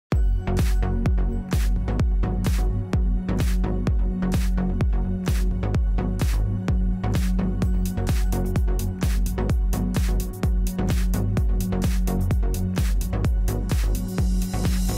Drill Dust Collector Demo 2021- Does it work?
Drill Dust Collector can effectively prevent dust diffusion and inhalation to protect your lungs.